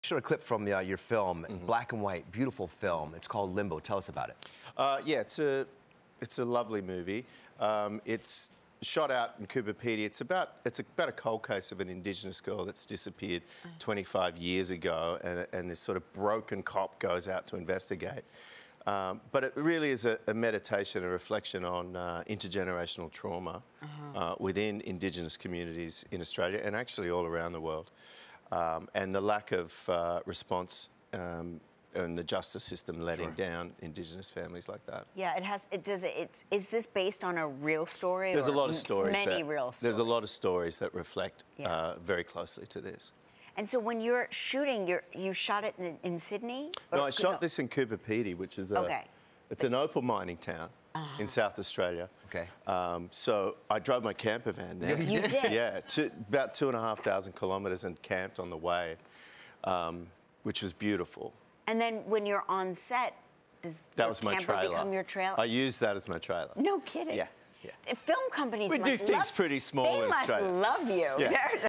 Simon Baker Talks About Filming "Limbo" in Australia
Simon Baker talks about his latest film "Limbo" about the Australian indigenous community.